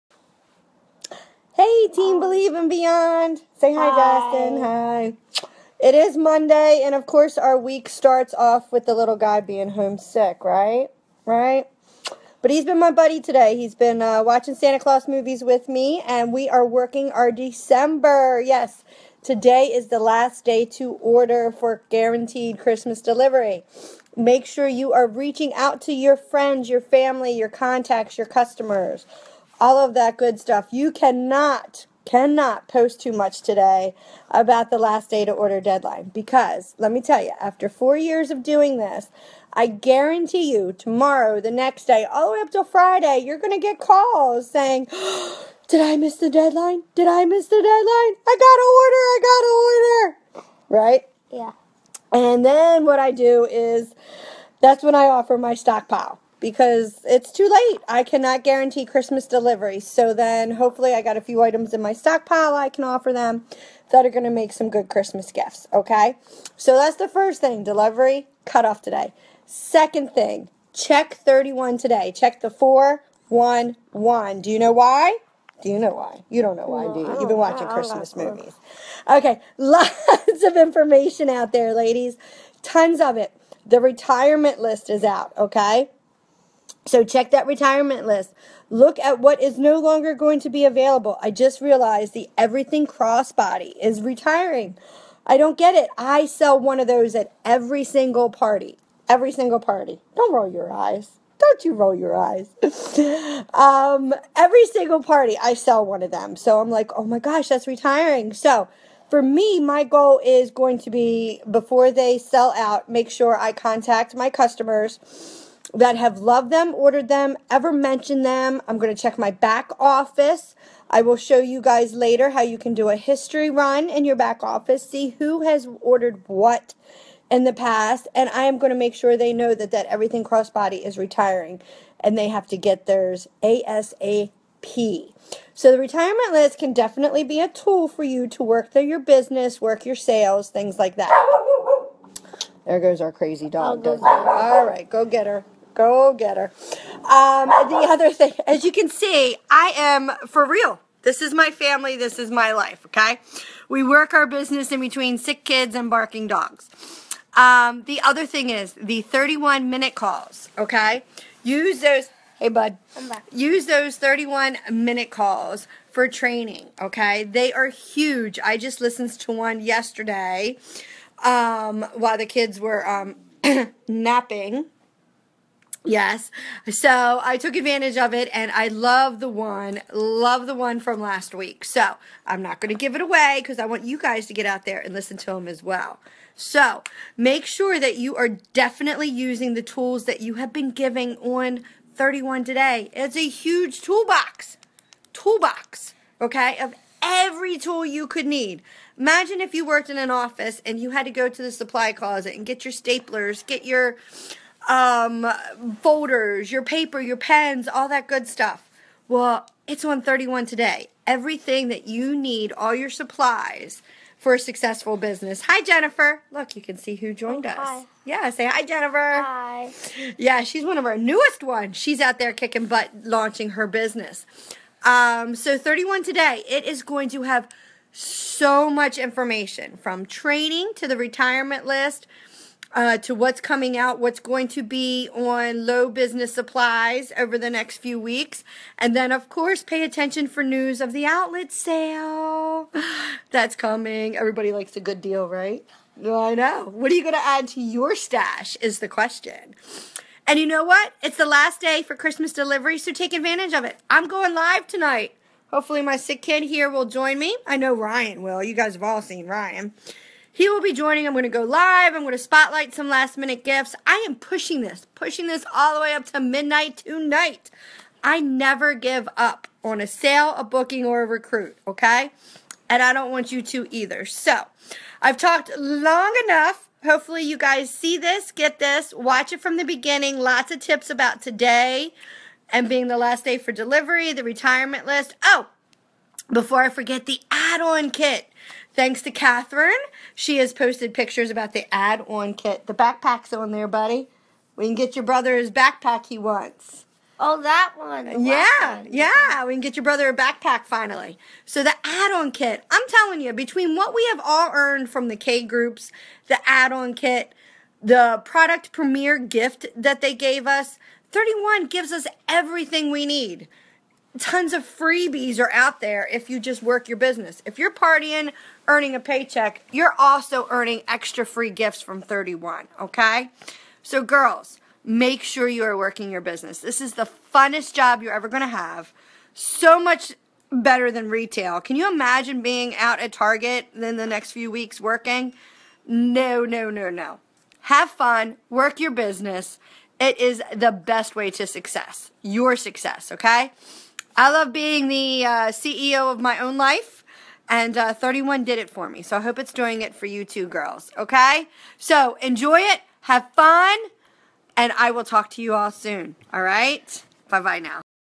Tips for December - Team Believe and Beyond
December is a short month with the delivery cutoff, but so much more you can still do to work your business! You don't want to be struggling in January!